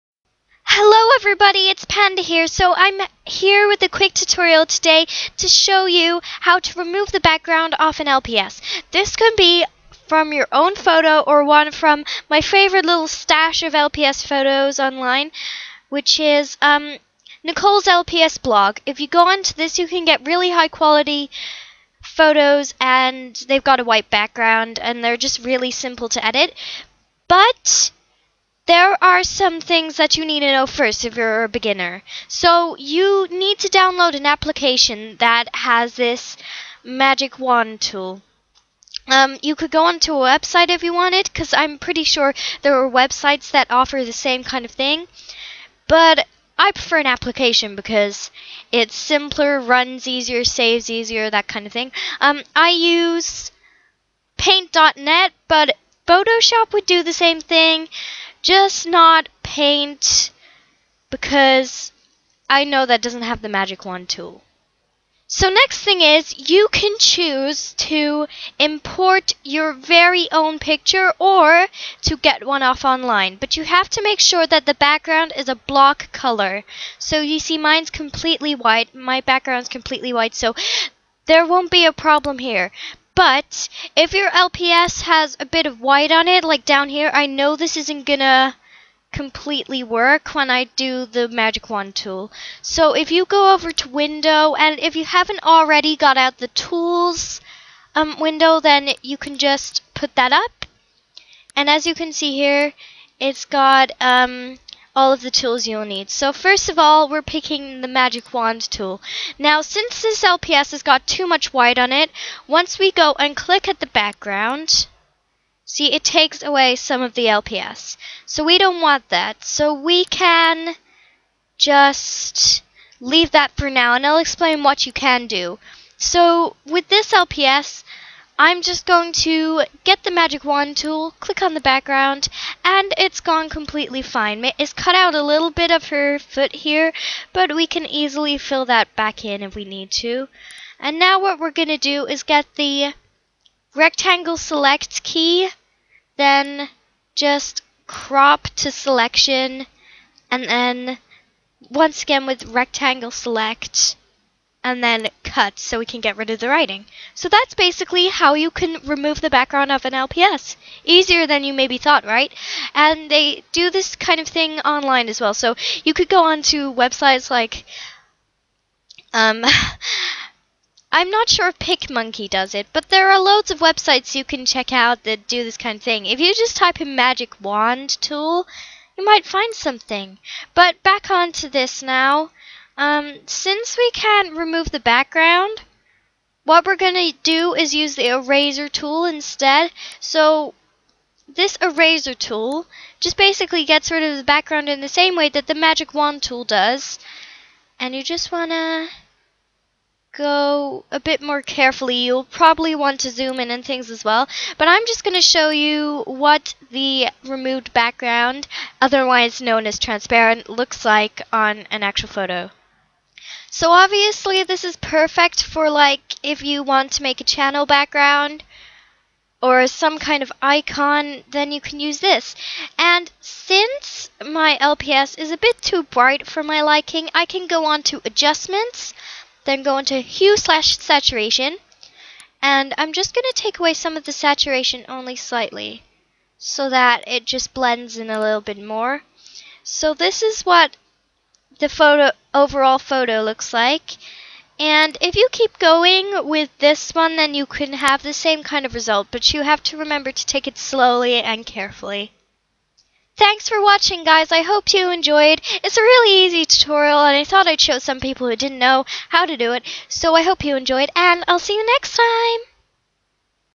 LPS: Editing Basics #1 (Removing The Background)
SKIP TO 1:32 FOR MAGIC WAND TUTORIAL.

Hey guys, it's Panda! I'm starting a new quick series demonstrating how to edit LPS photos. There are a few techniques to master if you want to make a channel art or icon, etc. Here's how to remove the background!

REMEMBER: If the LPS is white, then the Magic Wand tool won't work. If you don't want to use the eraser tool then you can take a photo yourself with a block-coloured background.

Sorry for talking loads! I just wanted to make things clear for beginners watching the video!

To film this video, I used:
-Hypercam
-Logitech Headset
(This setup is great for filming Minecraft videos too!)